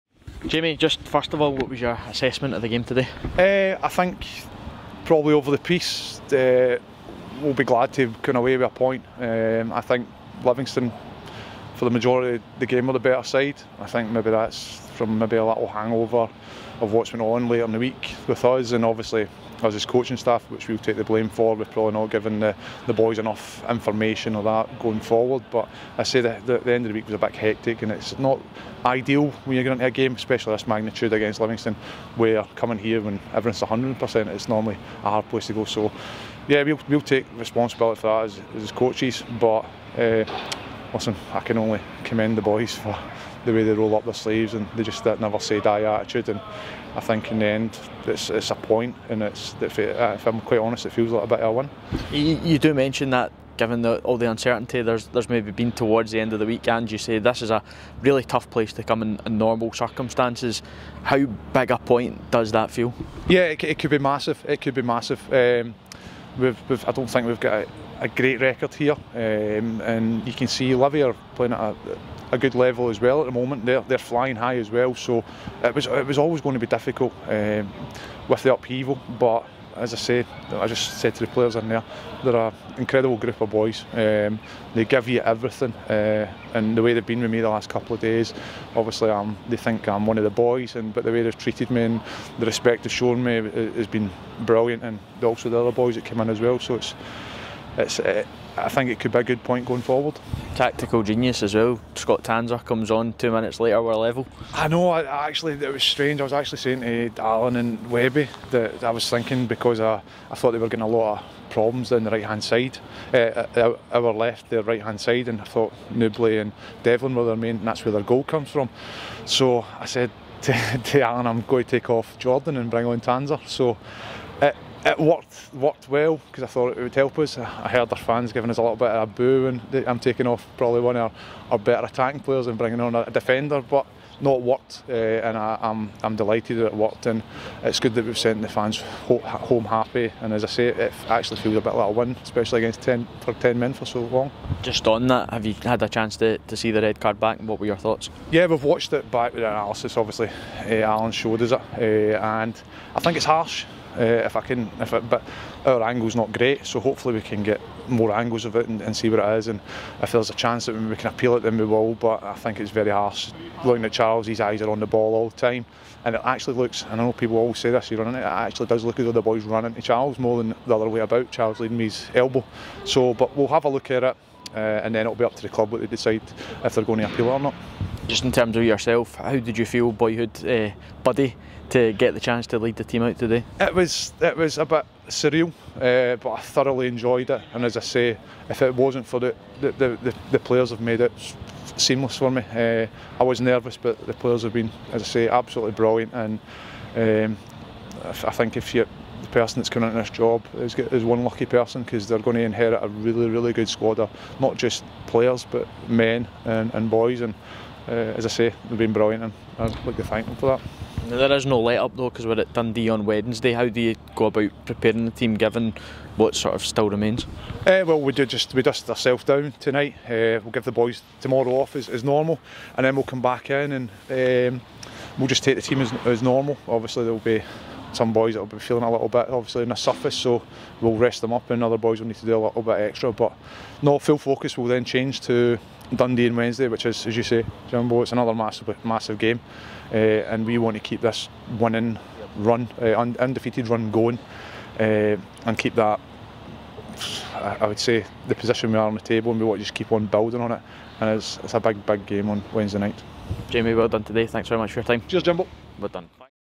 REACTION | Jamie Langfield Post-Livingston
Jamie Langfield gave his reaction after taking the team in today's cinch Premiership match away to Livingston.